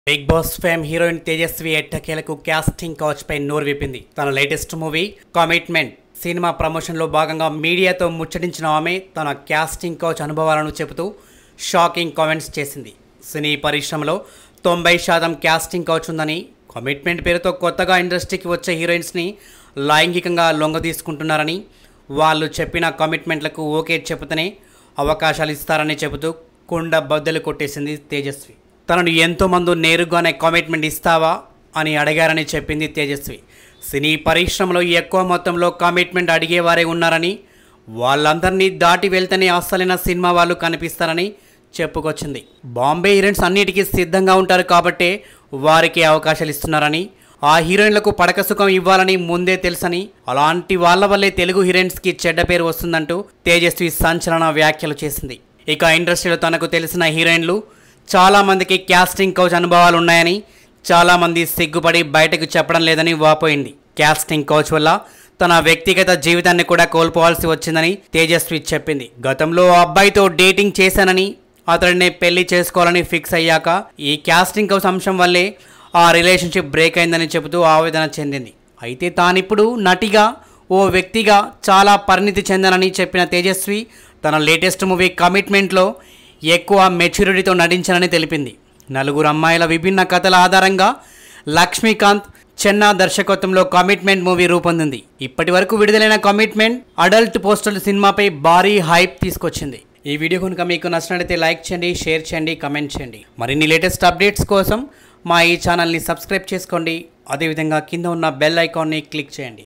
పడుకుంటేనే సినిమాలో అవకాశాలు ఇస్తారు | Tejaswi About Casting Couch | Telugu News | NN Media Trending
watch : పడుకుంటేనే సినిమాలో అవకాశాలు ఇస్తారు | Tejaswi About Casting Couch | Telugu News | NN Media Trending